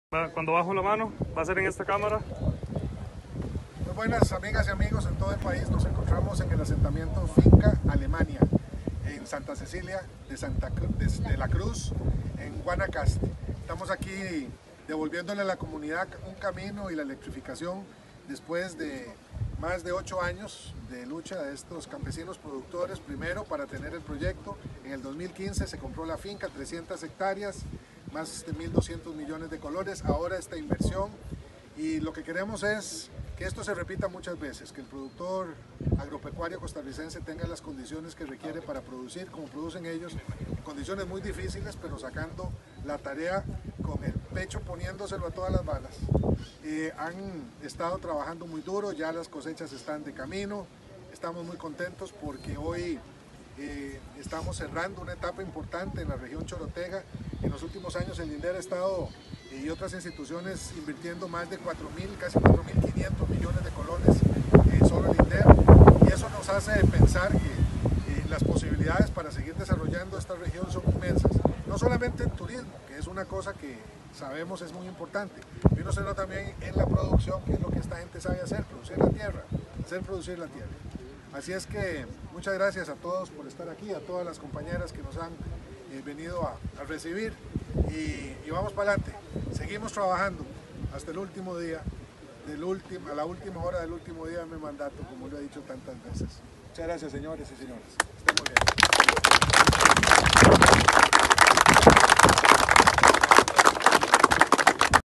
Caminos y electrificación en asentamiento campesino Alemania en Santa Cecilia de La Cruz
Estoy en el asentamiento campesino Alemania en Santa Cecilia de La Cruz, Guanacaste, donde entregamos caminos y electrificación.

Desde el 2015 el INDER ha invertido, sólo en el asentamiento, 1500 millones en caminos, parcelas, insumos, asistencia técnica, compras de finca, créditos rurales y electrificación .

Y en 4 años toda la Región Chorotega ha recibido inversión en infraestructura rural por ₵4.542 millones.